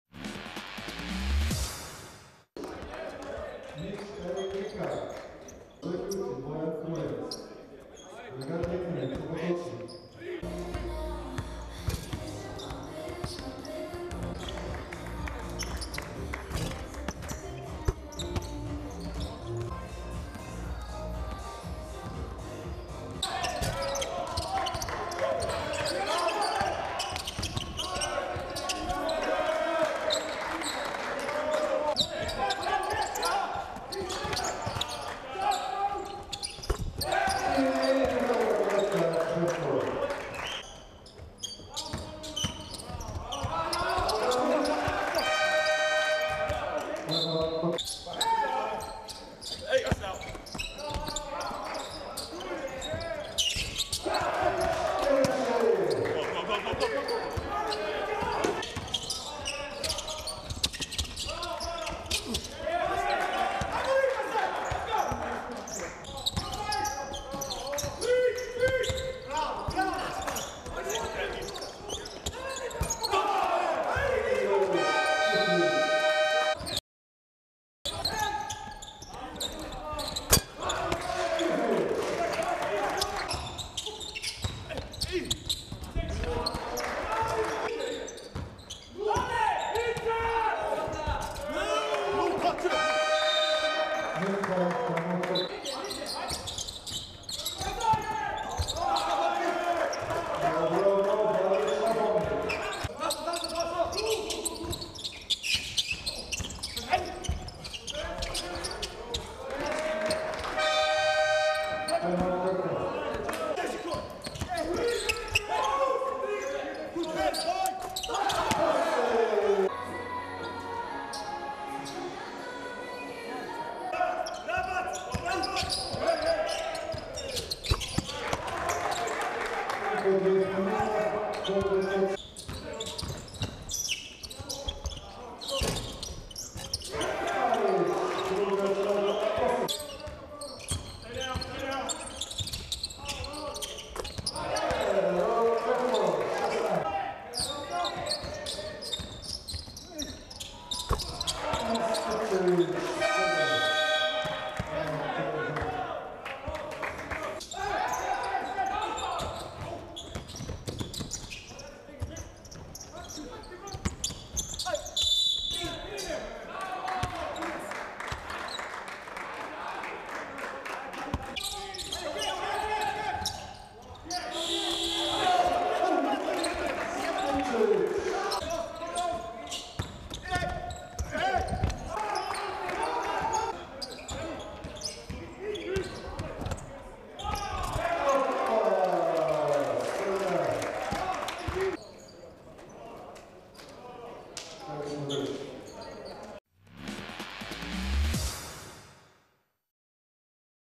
ABA Liga 2020/21 highlights, Round 11: Krka - Partizan NIS (19.12.2020)
Take a look at the highlights of the 2020/21 season ABA League game between Krka and Partizan  NIS in Leon Štukelj Sports Hall, Novo mesto.